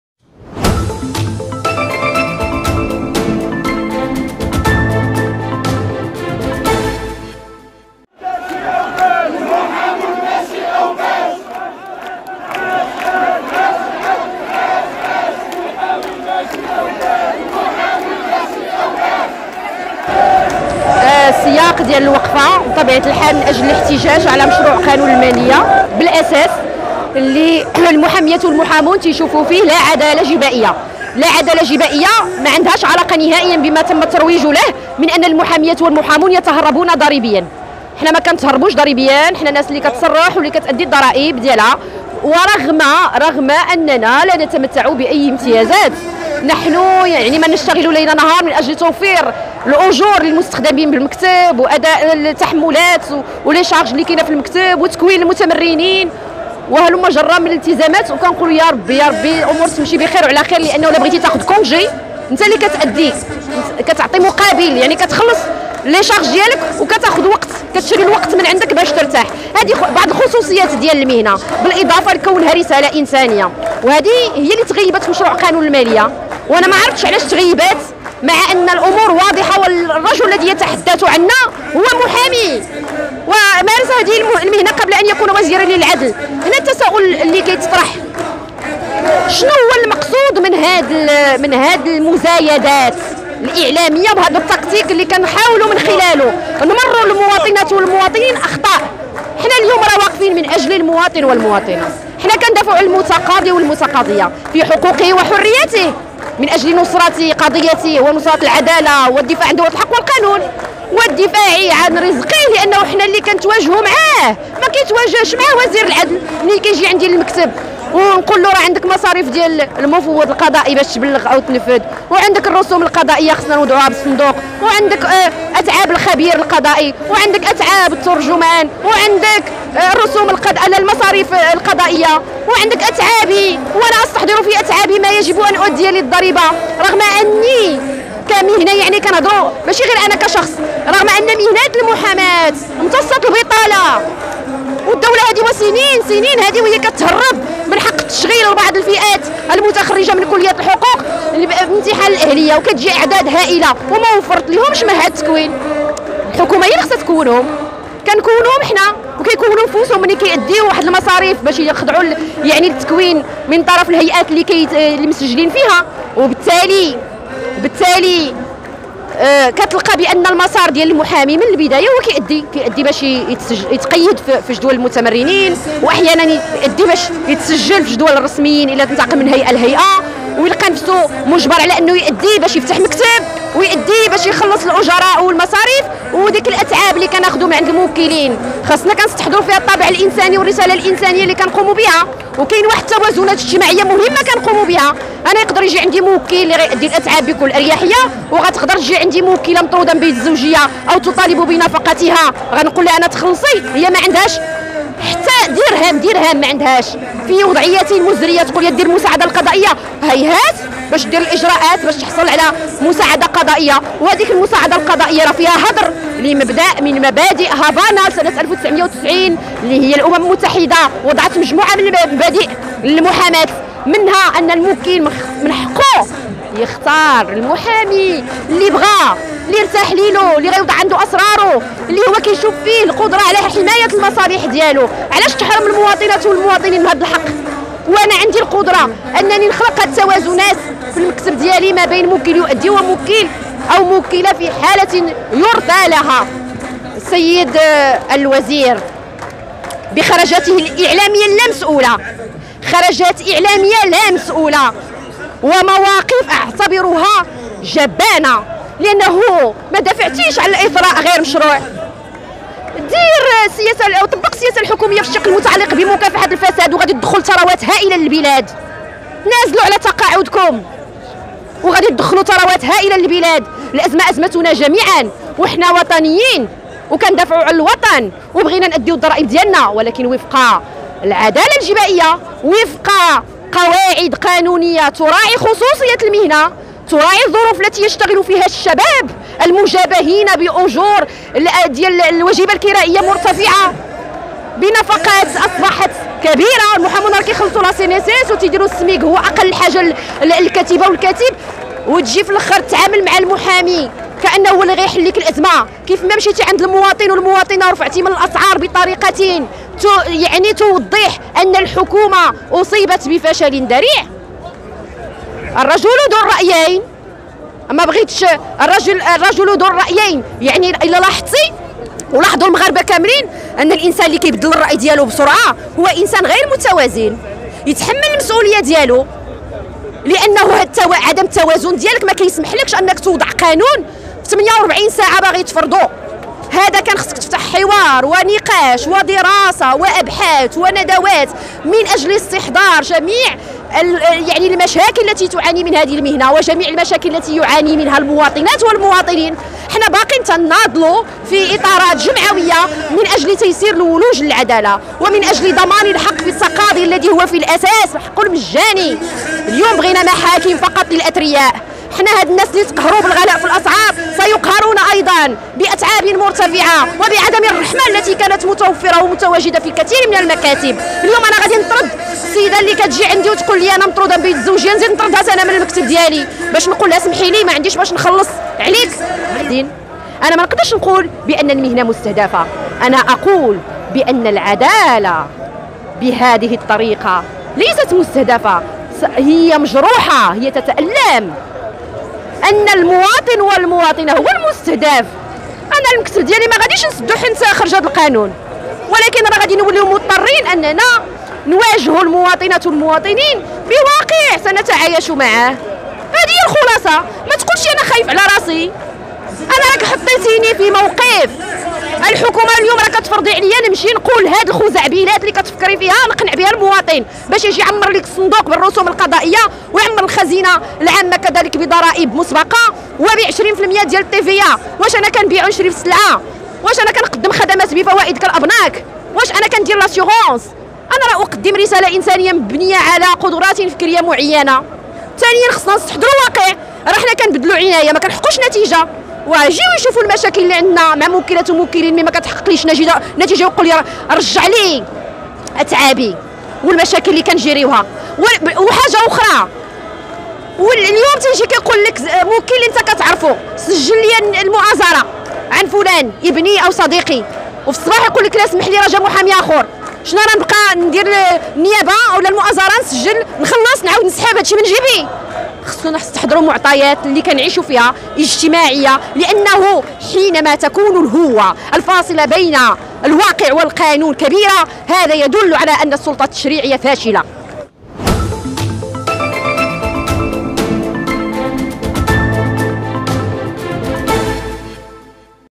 المحامية الإدريسي: العدالة في عهد الوزير وهبي مجروحة وتئن
انتقد المشاركون في وقفة احتجاجية للمحامين أمام البرلمان، وزير العدل عبد اللطيف وهبي، على خلفية مشروع قانون المالية برسم السنة المقبلة، وما يتعلق بالضرائب المفروضة على المحامين.
وأدان المحامون، خرجات وهبي غير المحسوبة في حق مهنة المحاماة وكافة المحامين، وإغلاقه "باب الحوار" و”القطع مع المقاربة التشاركية في الشأن المهني، داعين لمزيد من النضال حتى تحقيق مطالبهم التي تنسجم وحق المواطنين في الولوج للعدالة..